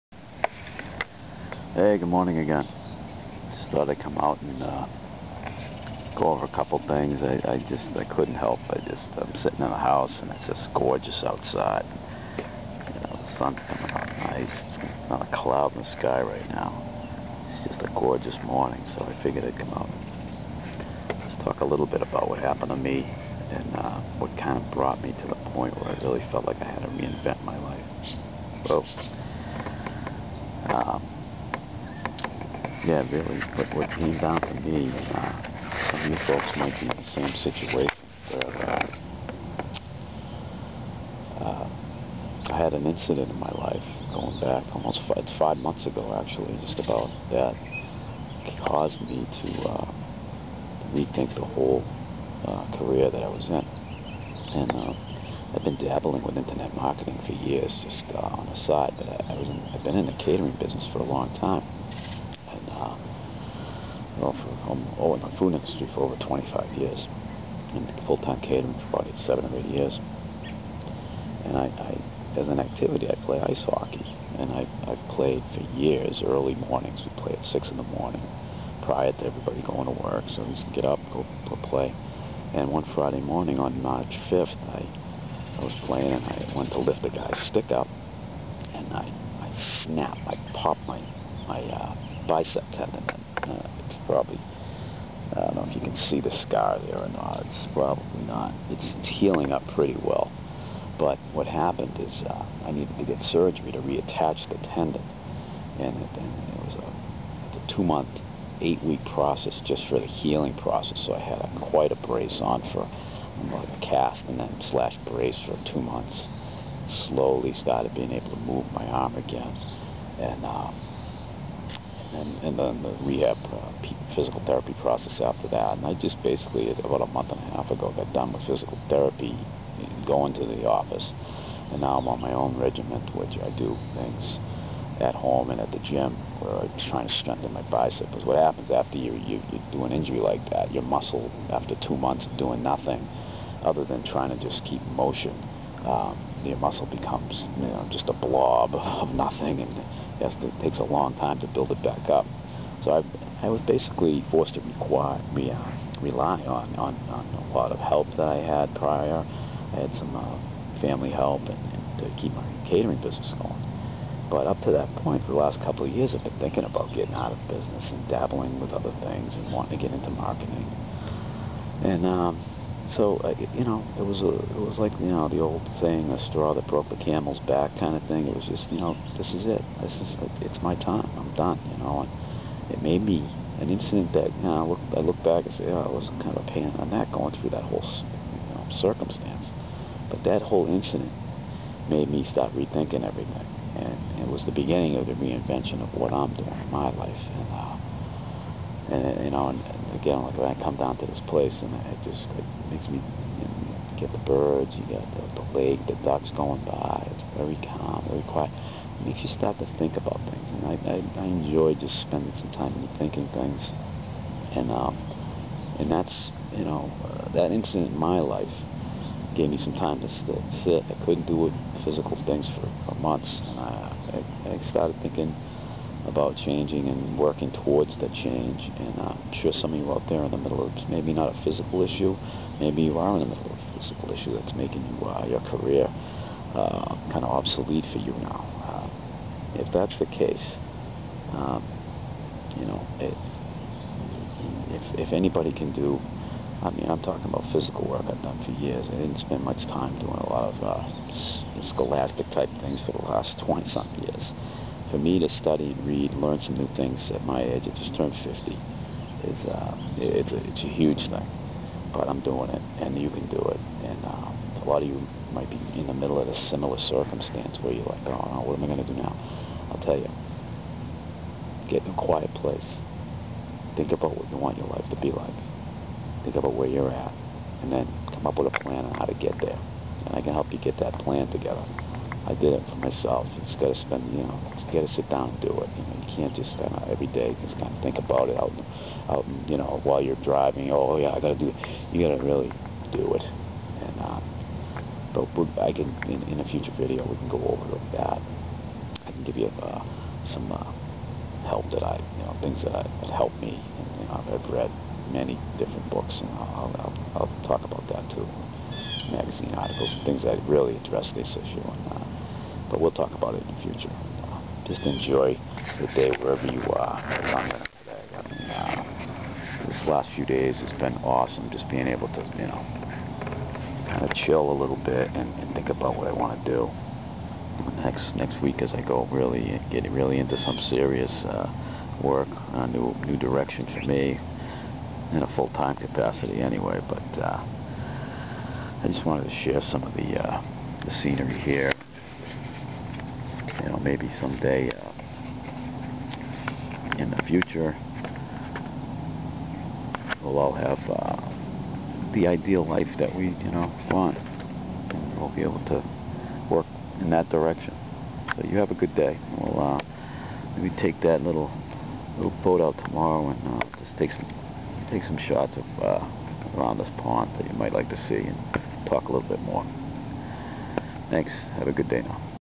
Reinvent Your Path 2
I'll tell you what happened to me to make me start the reinvention process.
Some times you need a kick in the butt !!